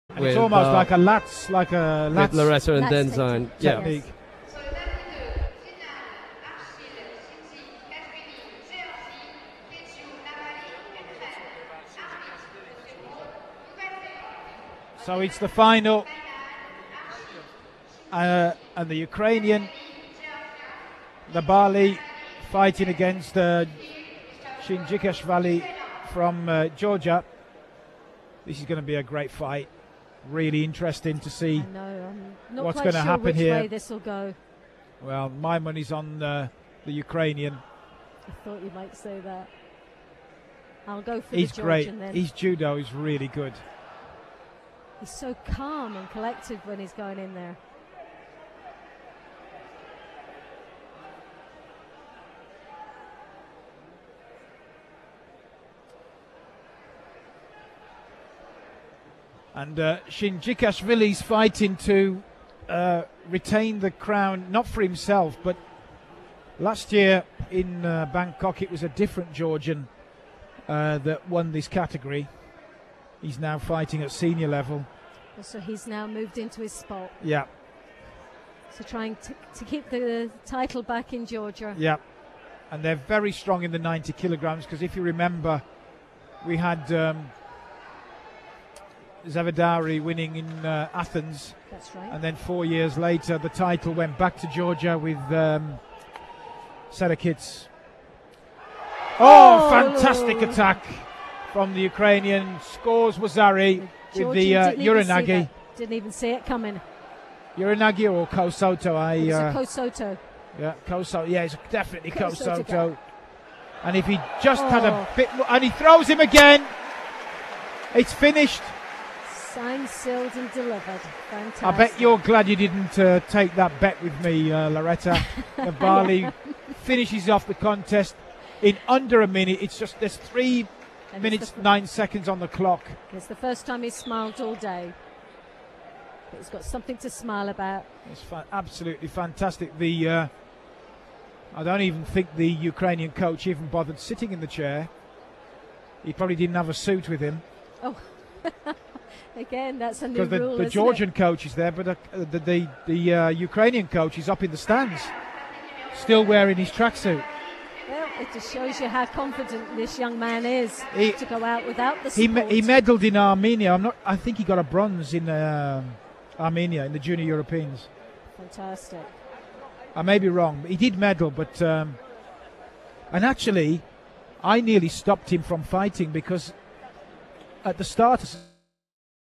Judo 2009 Paris: Shinjikashvili (GEO) - Nhabali (UKR) [-90kg].
Archil Shinjikashvili (GEO) - Quedjau Nhabali (UKR) [-90kg]. Judo 2009 World Championship Juniors Paris